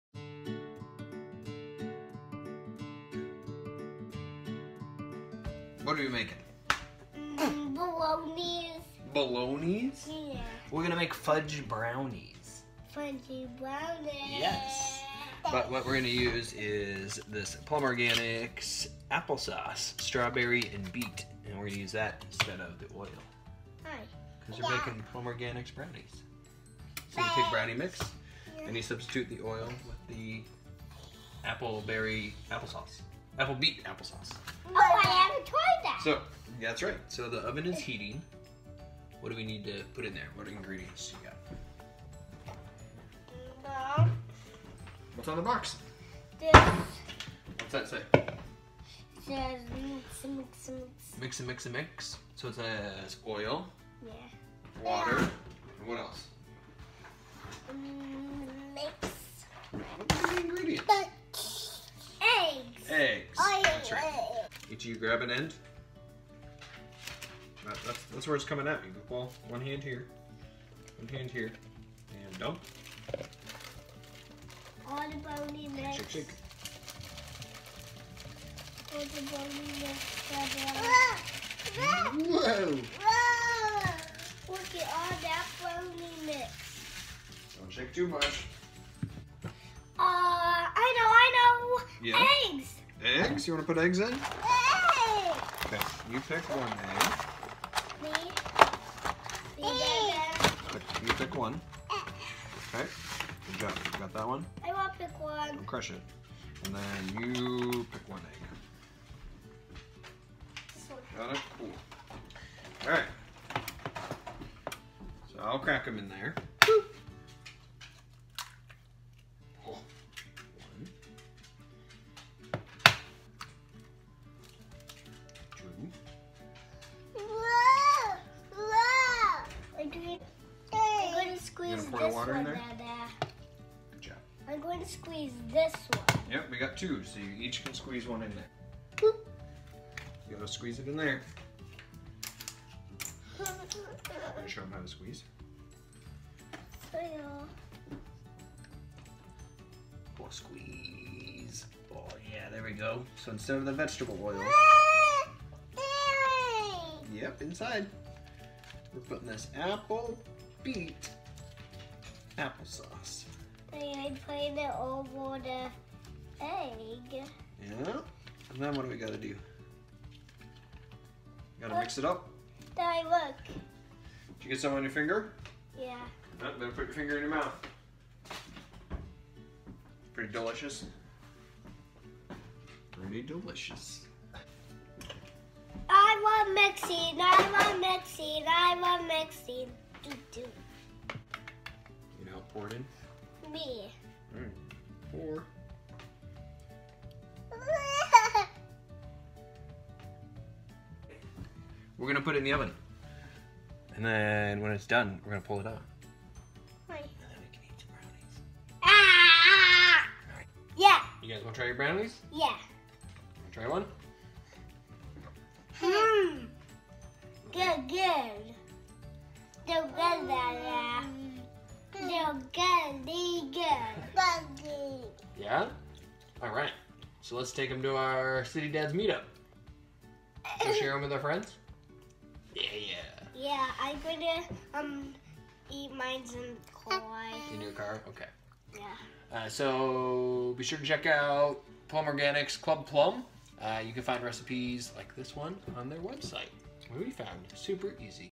Plum Organics Strawberry Beet Brownies
Whipped up a batch of Plum Organics Brownies for our City Dads Meetup